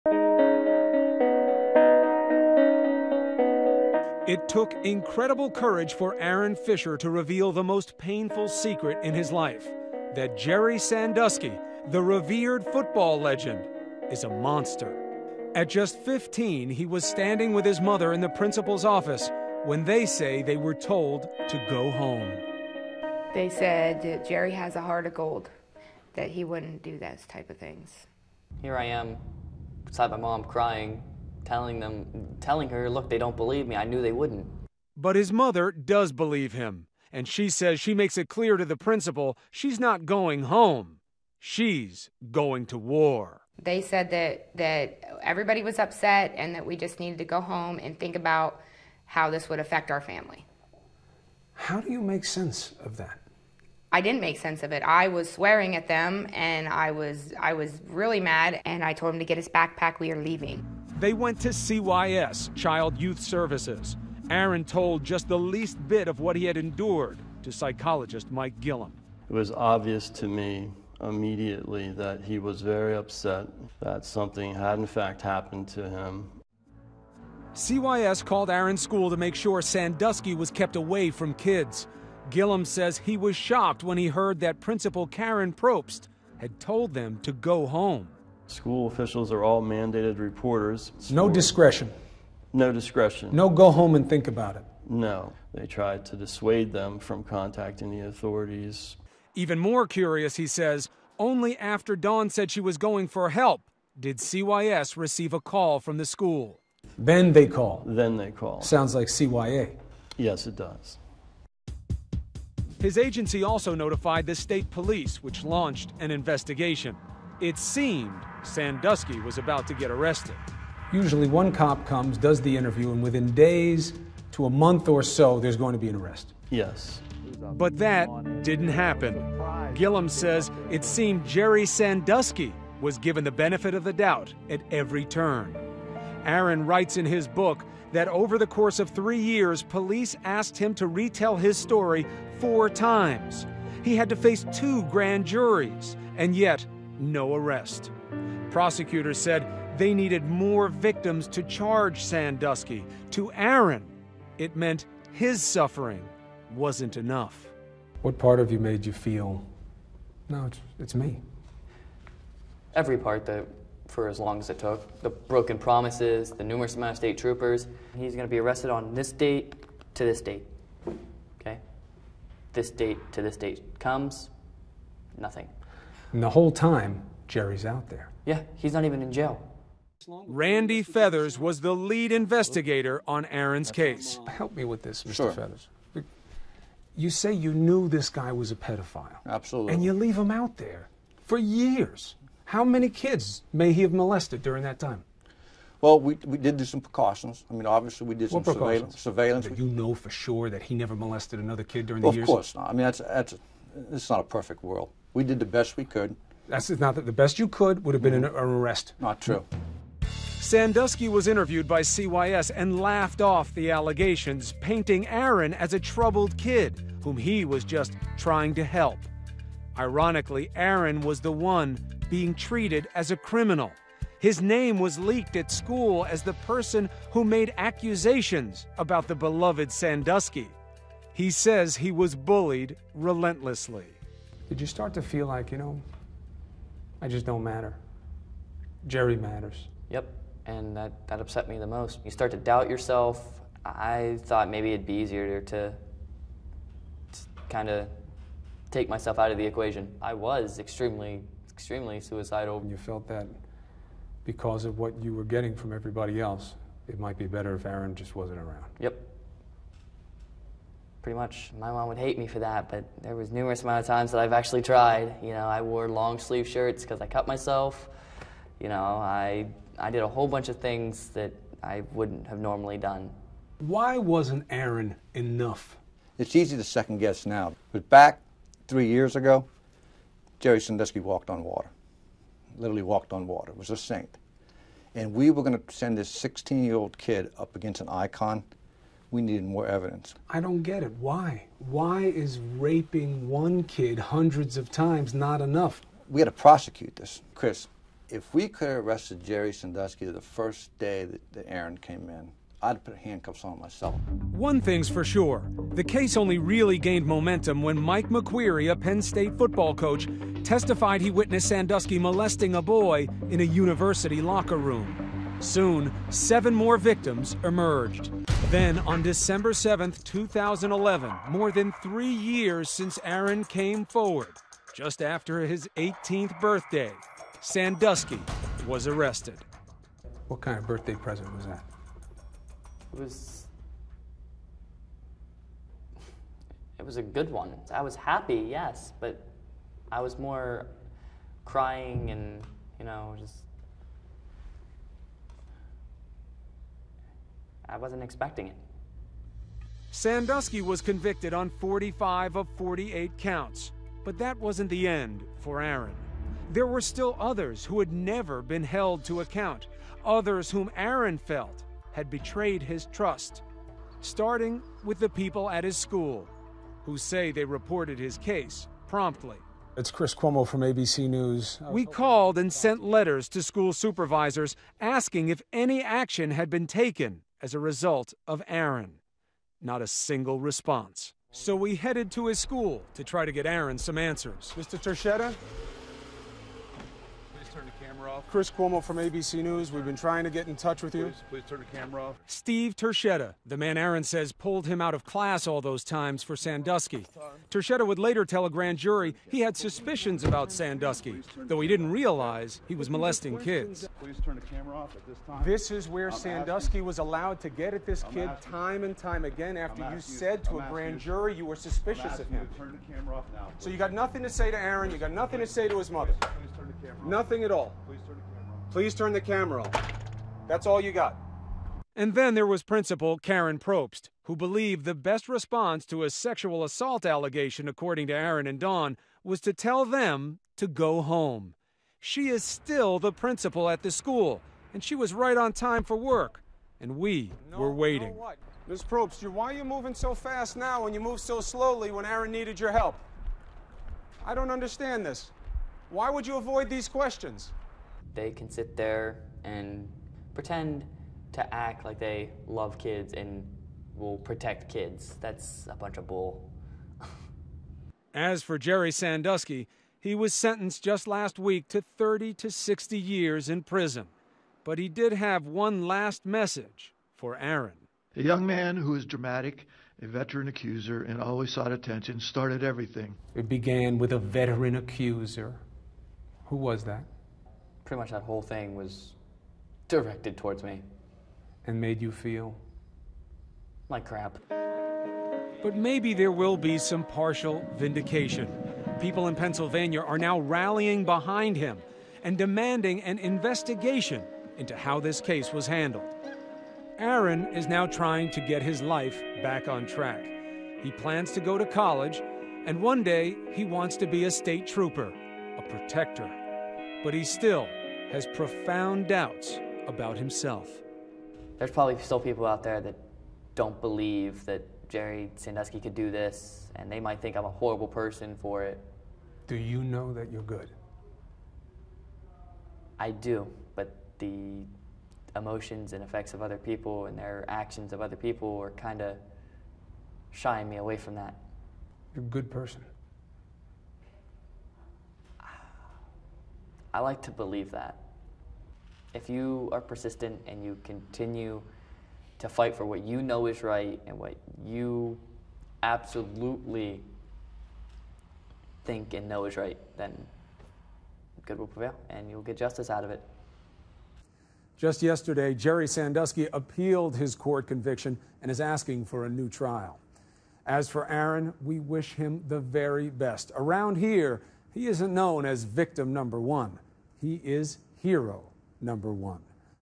Sandusky Victim 1: Lonely Fight for Justice
Part 2: Aaron Fisher says school officials let him down.